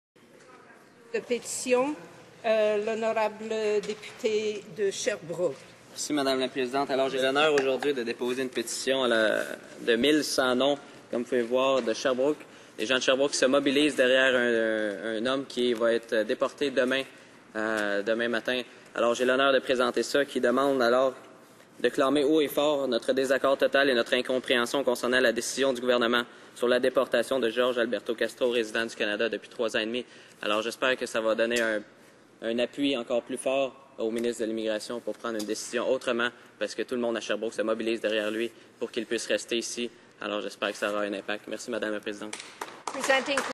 Le député Dusseault présente une pétition contre la déportation de Jorge Alberto Castro
Aujourd'hui, le député néo-démocrate Pierre-Luc Dusseault (Sherbrooke) a déposé à la Chambre des communes une pétition signé par 1 100 Sherbrookois qui réclame le renversement de l'avis d'expulsion de Jorge Alberto Castro.